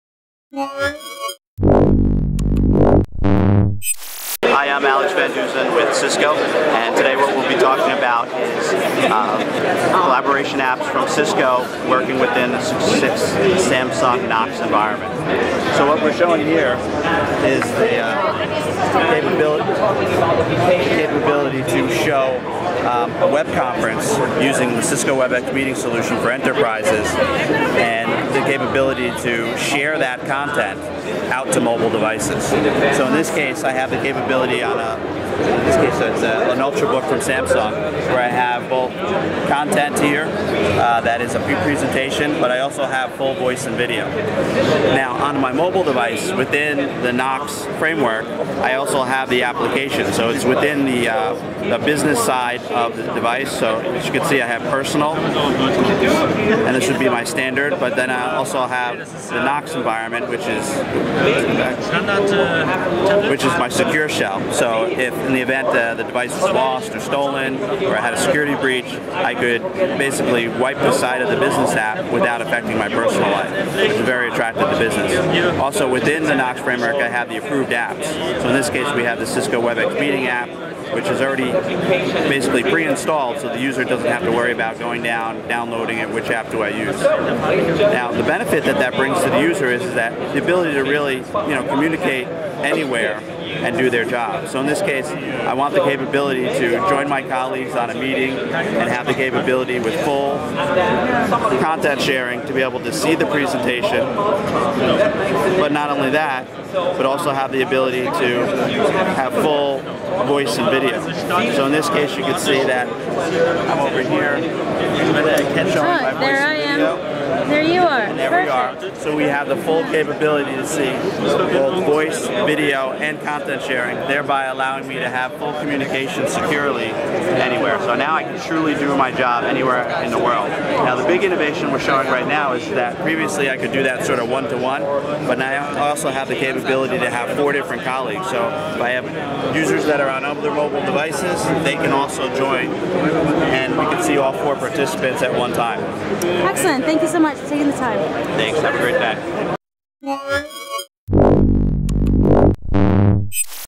Samsung SAFE with Knox Demo at MWC 2013
samsung safe and knox At Samsung's Mobile World Congress 2013 booth, we got a chance to see the KNOX container running on a Samsung Galaxy S III. By simply tapping the KNOX icon on the home screen, a rep showed us how the phone prompts you for a password then opens a separate environment complete with different wallpaper and a tiny KNOX logo on top of each app.